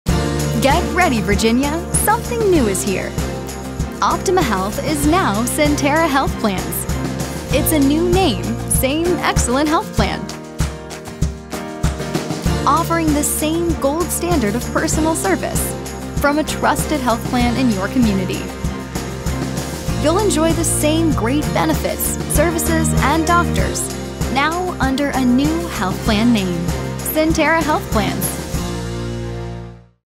Optima Health is now Sentara Health Plans
We’re thrilled to announce that Optima Health is rebranding to become Sentara Health Plans! This transition marks an exciting new chapter, reflecting our ongoing commitment to providing exceptional health insurance coverage and support.

We’re excited to continue serving you under our new name and look forward to the future with renewed energy and commitment.